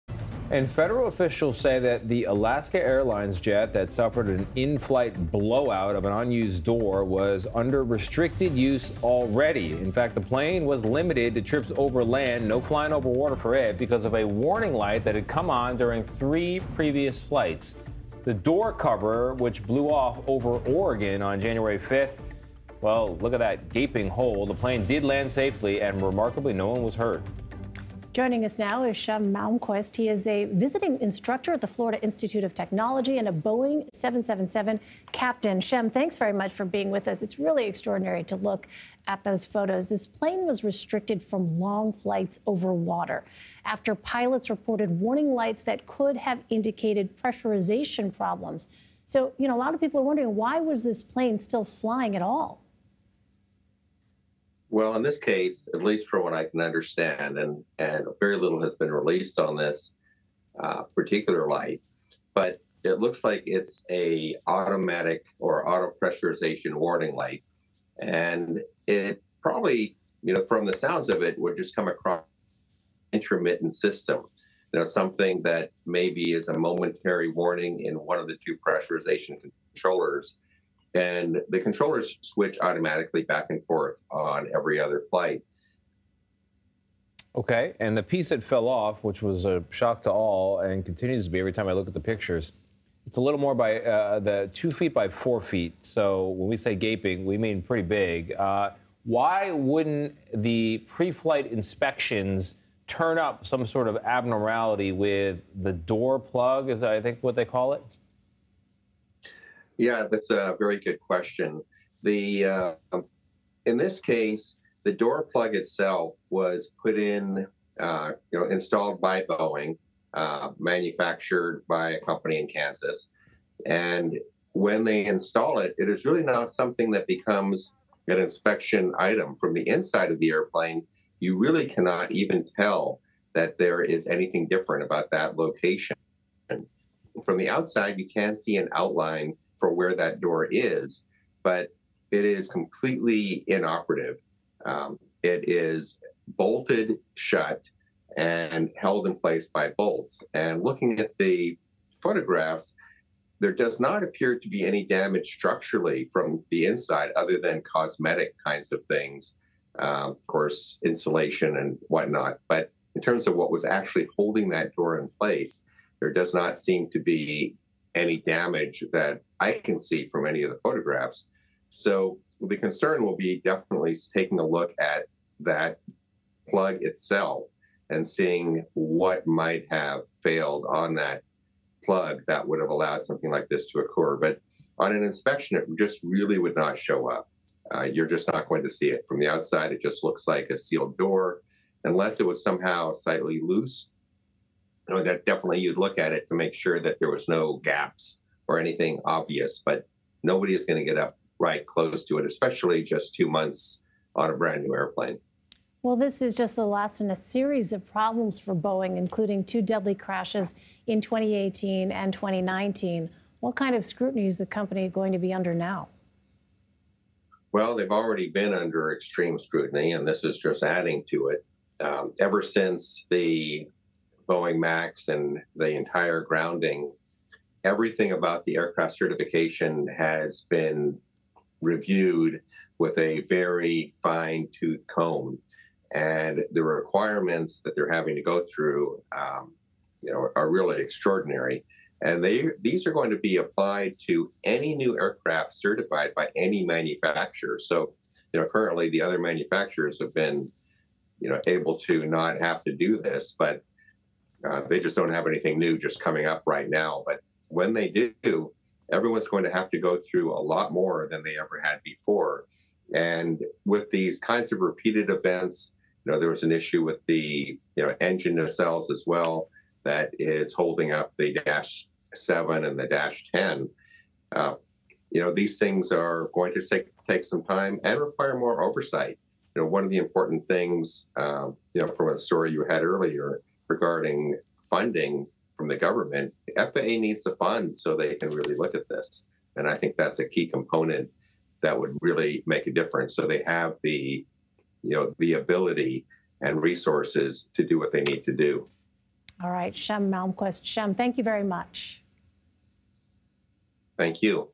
Investigation into Alaska Airlines plane's door plug blowout underway
A depressurization warning light may become a key point in the investigation into the mid-air door plug blowout that occurred Friday on an Alaska Airlines Boeing 737 Max 9. Shem Malmquist, a Boeing 777 captain and visiting instructor at the Florida Institute of Technology, joins CBS News with a look at the details of the incident that will be investigated.